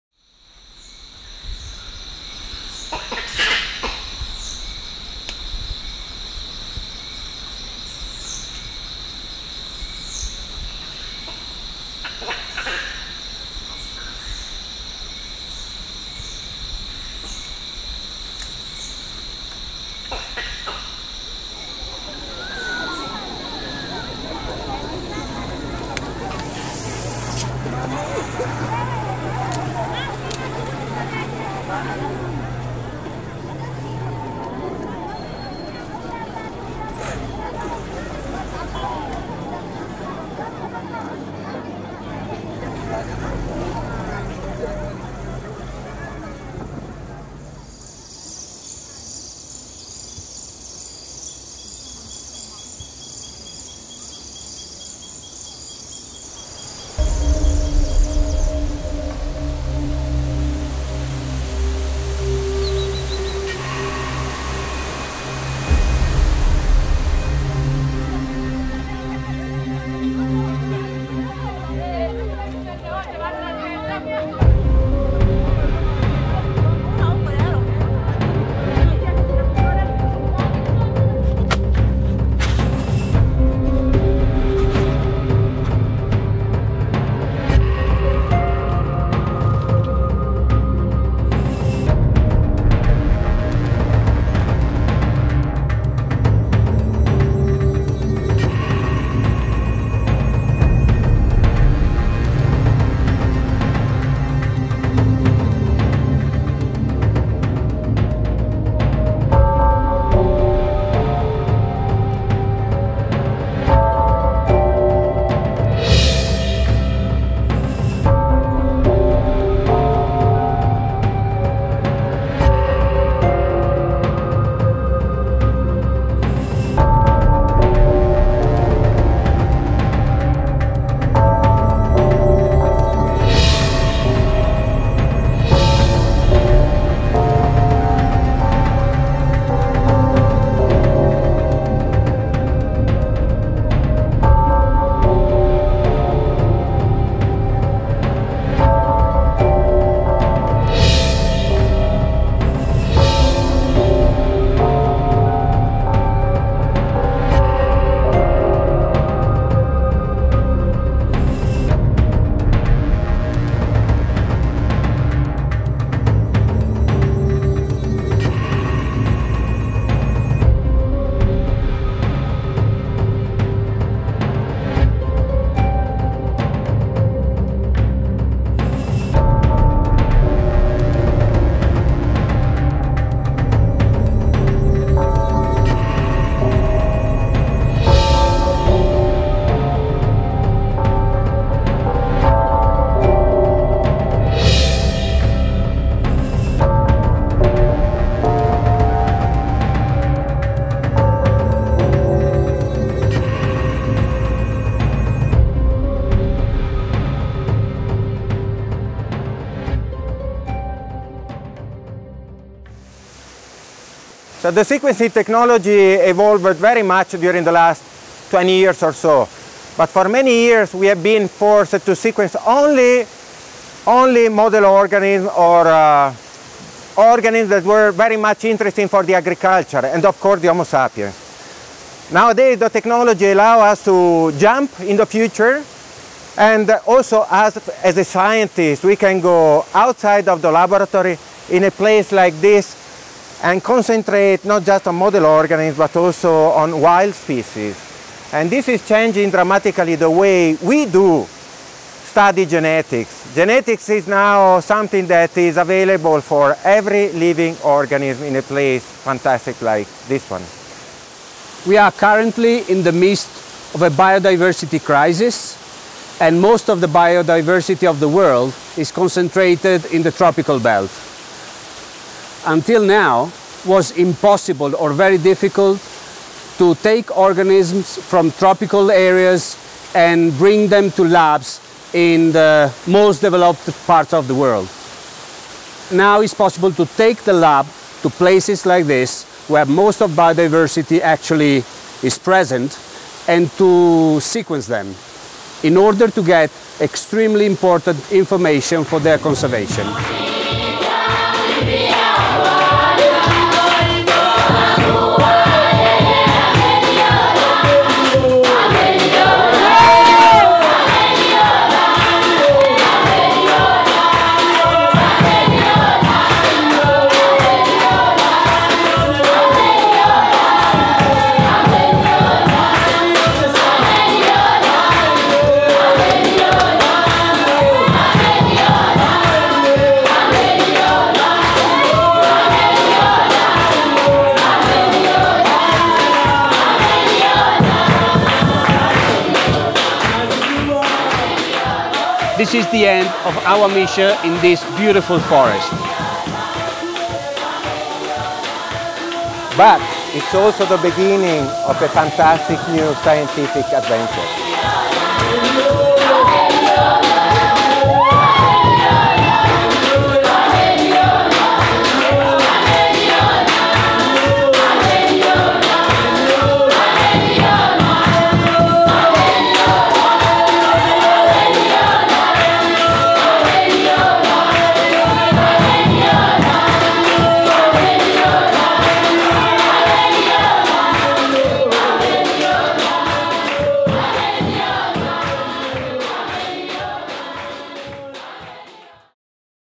Massimo Delledonne: MinIONs and Nanofrogs
Until today, the investigation of the biological diversity of many parts of the world has been obstructed by the inability to perform field based DNA analysis, especially in those regions where the biological richness is higher, like the countries in the inter-tropical area. Some years ago, Edward Osborne Wilson wrote that the possibility to sequence the DNA of organisms in the most remote places of the planet, making possible the identification of any organism, one day will allow a sensational step ahead in the study of biodiversity. On May 4th, 2015 a team of Italian researchers performed the first genetic tests on the slopes of an extinct volcano in the forests of southern Tanzania, making Wilson’s dream a reality. Using the MinION device, they sequenced the DNA of a wild frog species and transmitted the raw data via a standard smartphone to Oxford Nanopores cloud based application, Metrichor. Metrichor performed a custom automated workflow that allowed the team in Tanzania to determine in real time the identification of the species of frog that they had just sequenced. The data was simultaneously sent to their colleagues at the MUSE of Trento where they were able to participate in the discovery.

Flongle, GridION, MinION, MinIT, PromethION, and VolTRAX are currently for research use only.